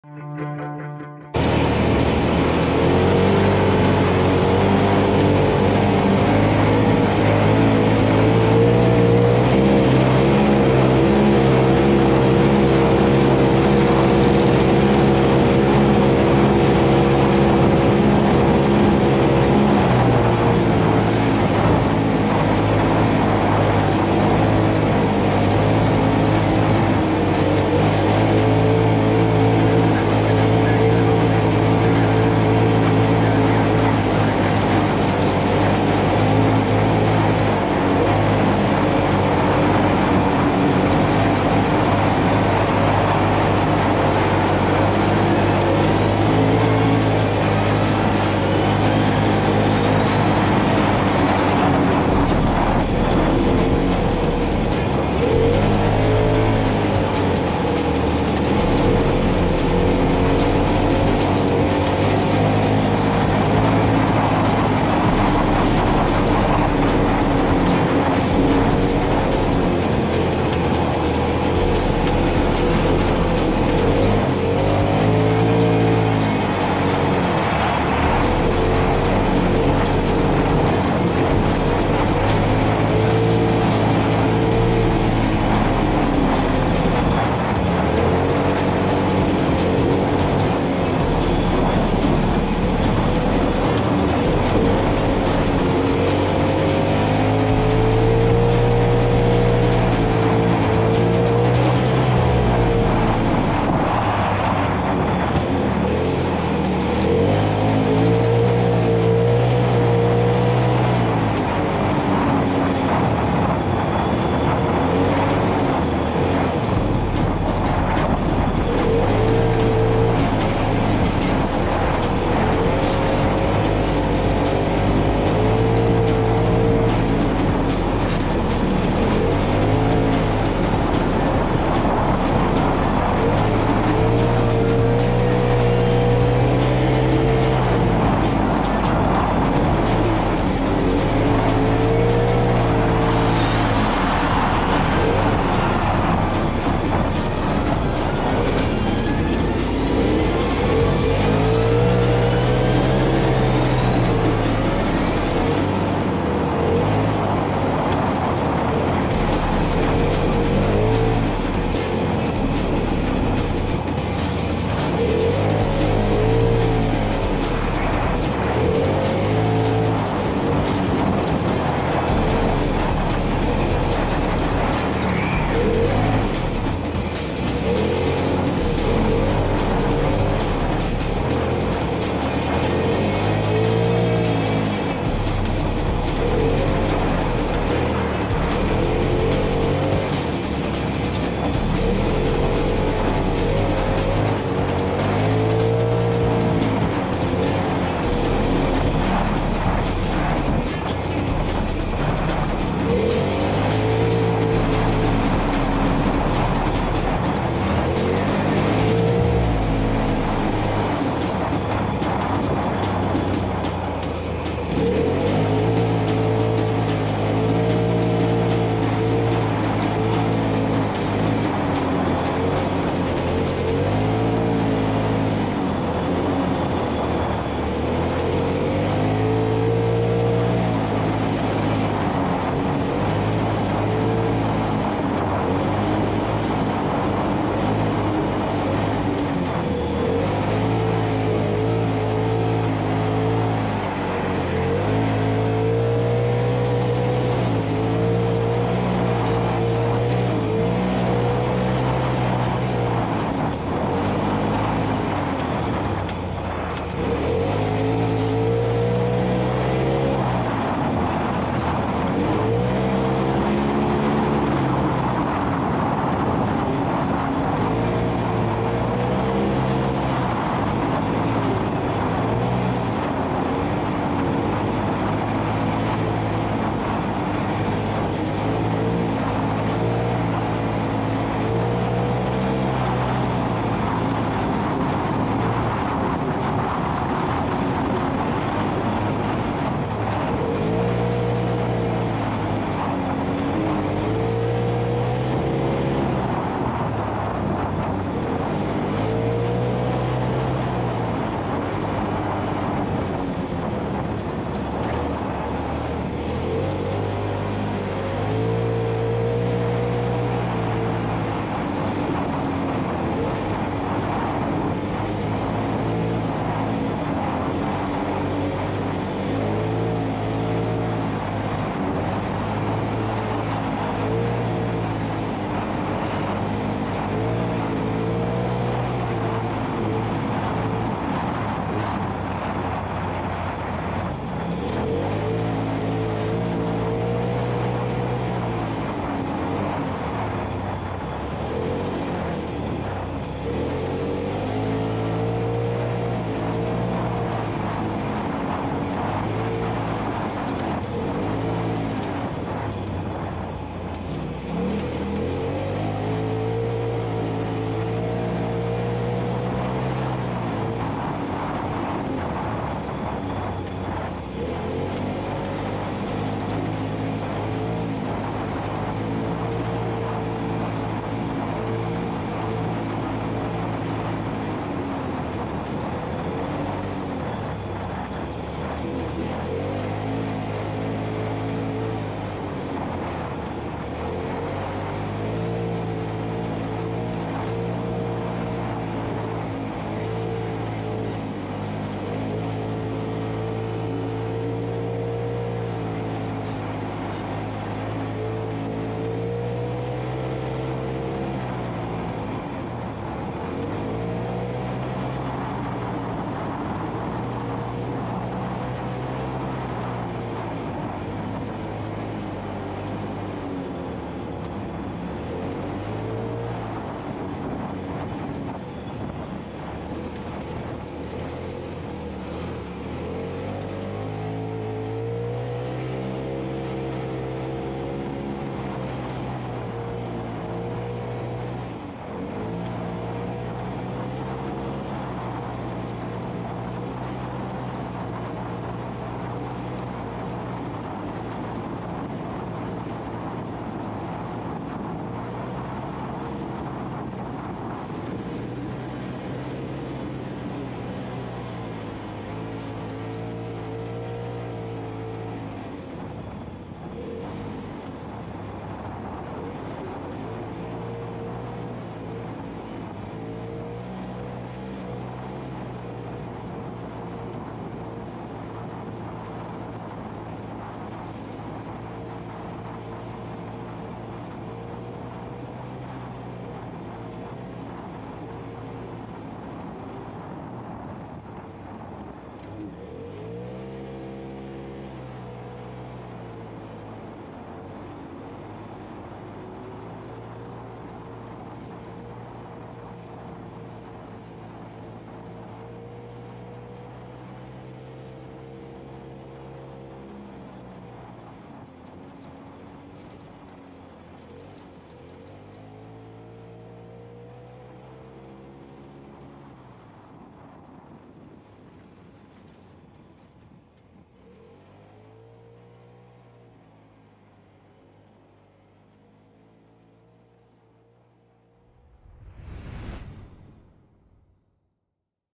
Glamis Sand Dunes Riding.
Awesome sand dunes riding with friends